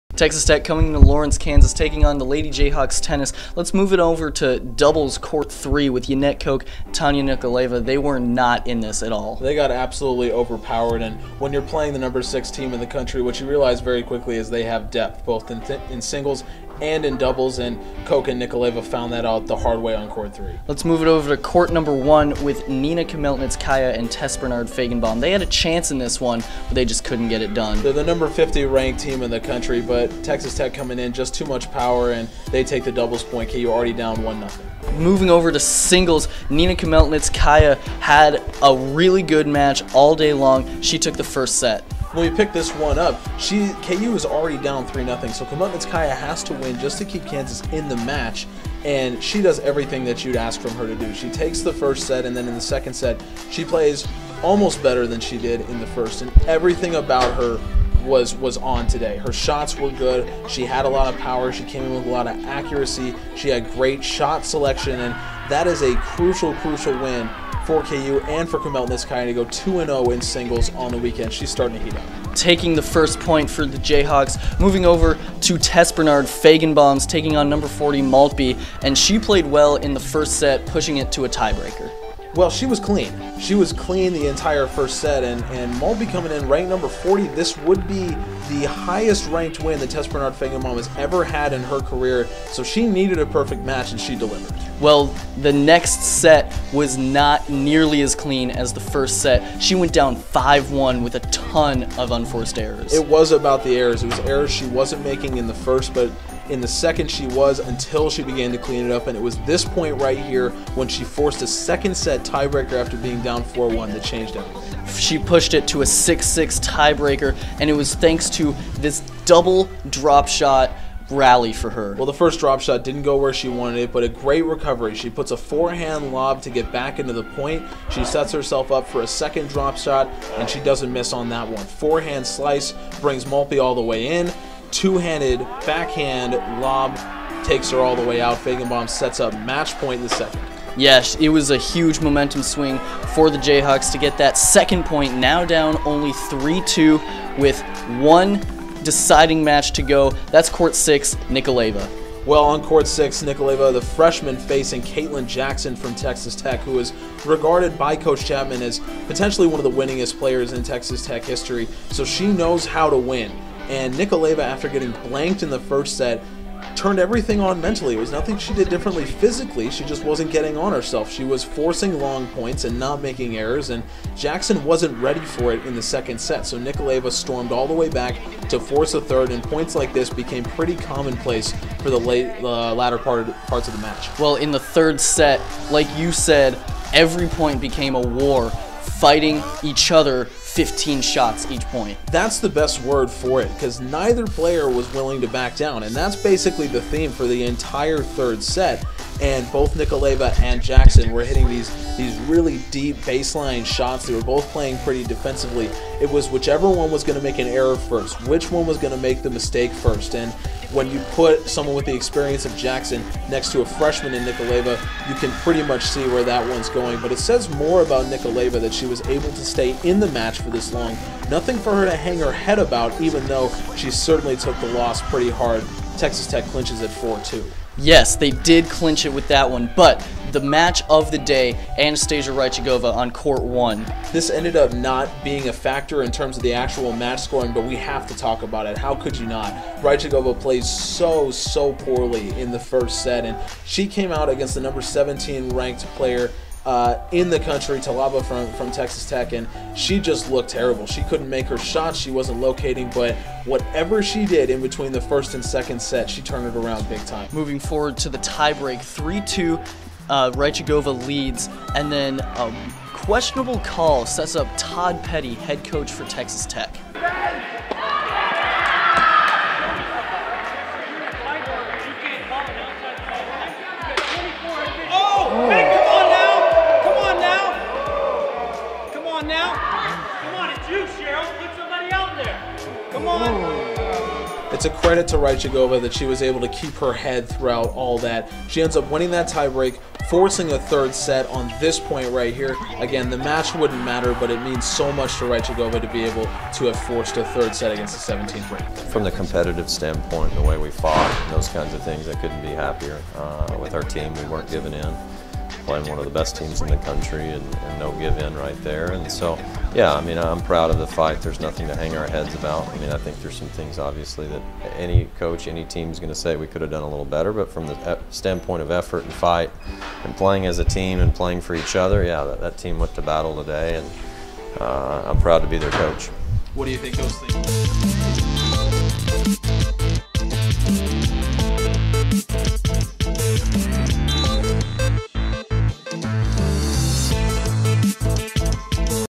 KU Tennis in a tough Battle Against #6 Texas Tech
KU takes on #6 Texas Tech in what would become one of the best matches of the season so far. Featured: KU fighting with its back against the wall, Khmelnitckaia continues her hot streak and Rychagova stages miraculous comeback against the #17 player in the country.